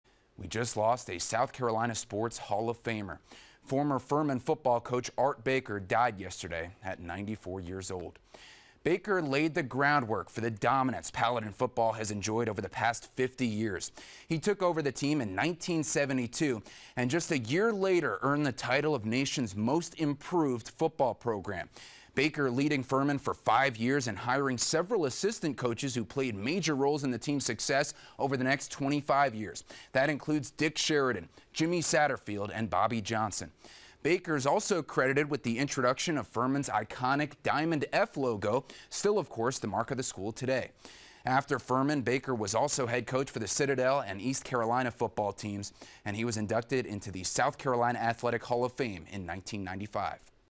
Art Baker dies
Art Baker laid the groundwork for the success of the Paladin Football team beginning in 1972.